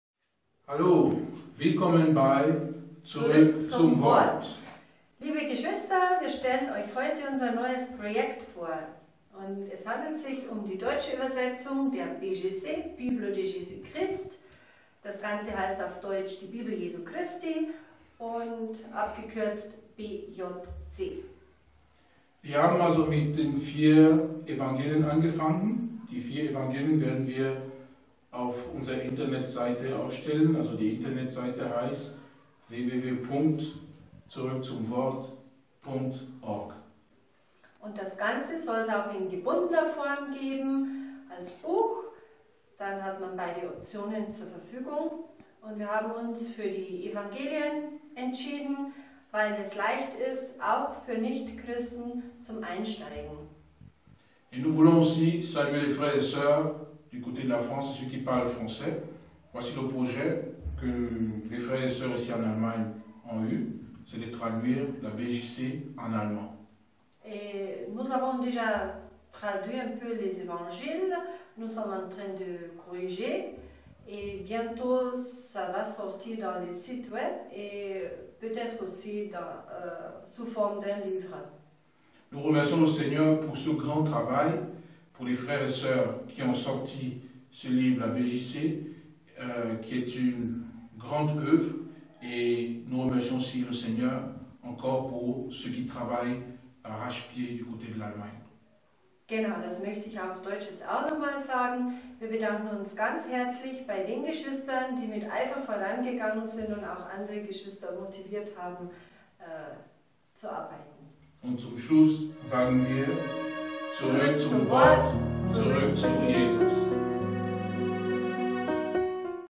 Projekt "Bibel Jesu Christi"
Die deutsche Übersetzung der vier Evangelien Matthäus, Markus, Lukas und Johannes aus der französischen Bibelversion BJC („Bible de Jesus Christ“) erscheint in Kürze.